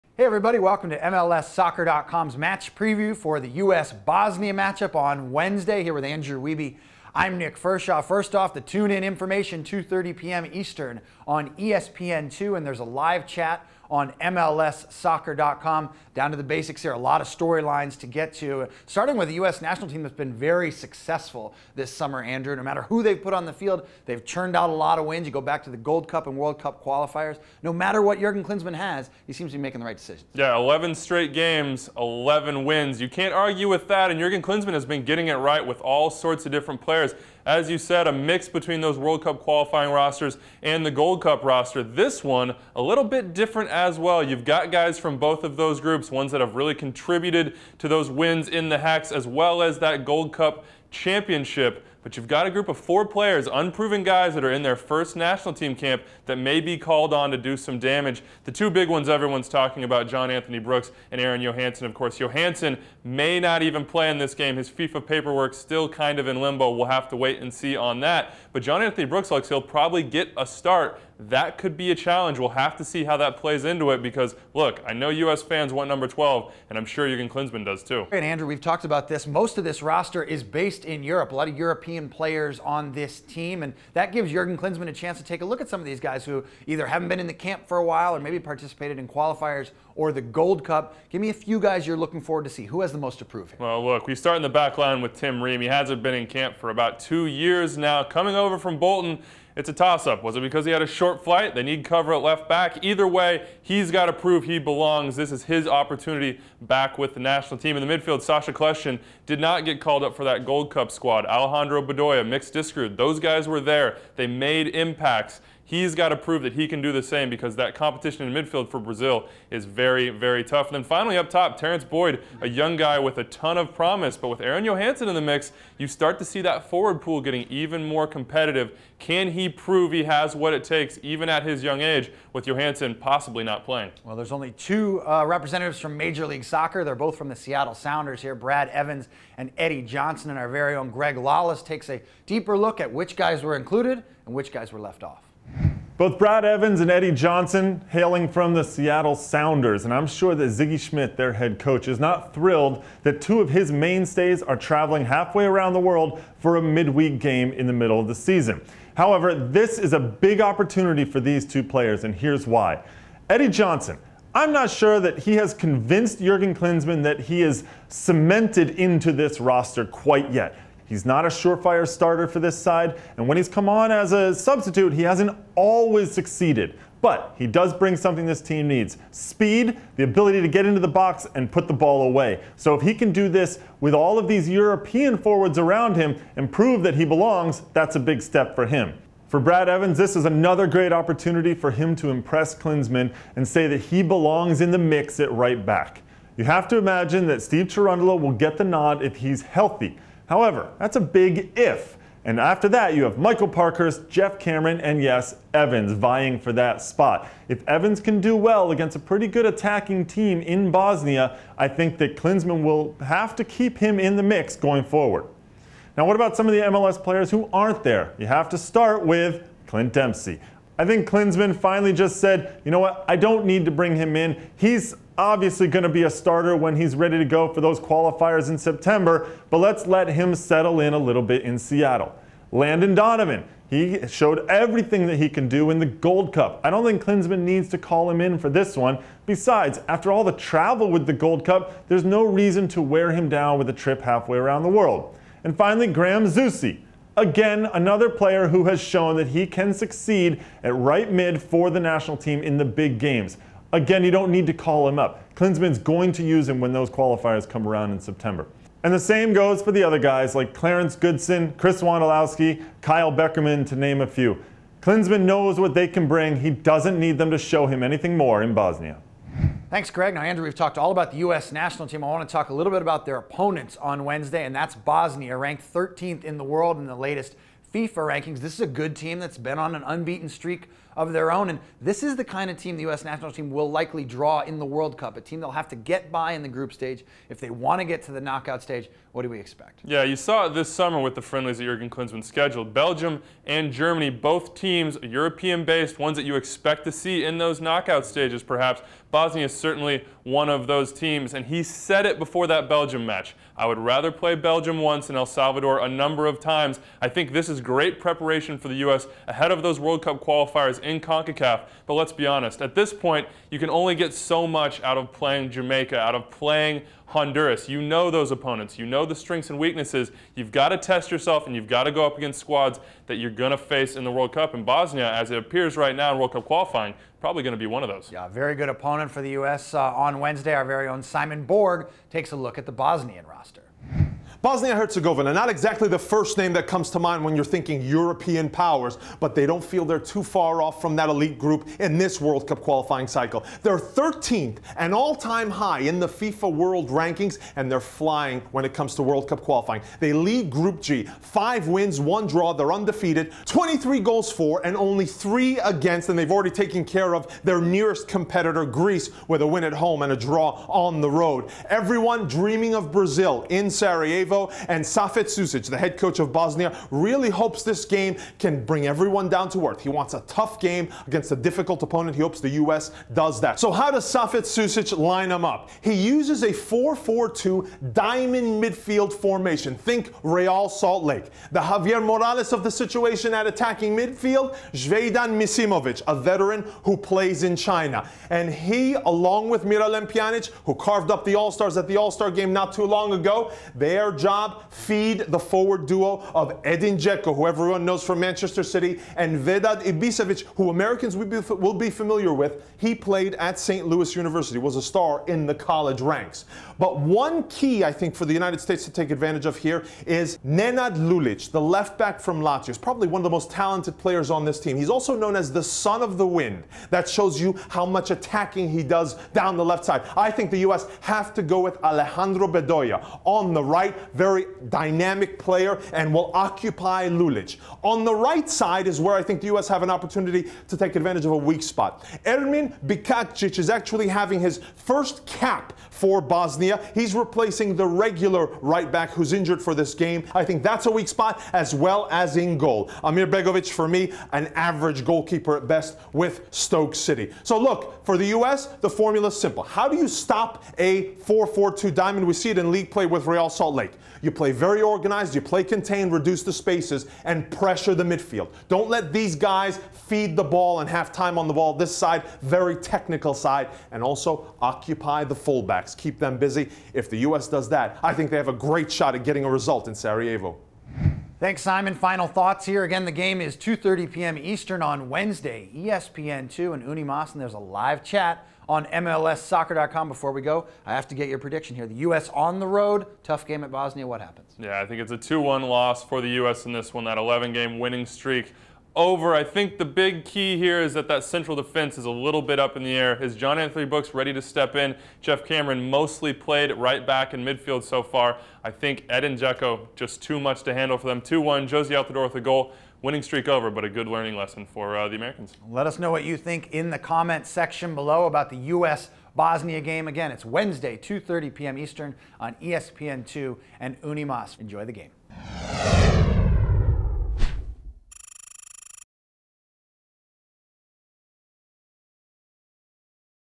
USMNT vs Bosnia-Herzegovina | Game Preview
What are your predictions for the friendly between the United States men's national team and Bosnia-Herzegovina. MLSsoccer.com gives you all the information you need ahead of the game.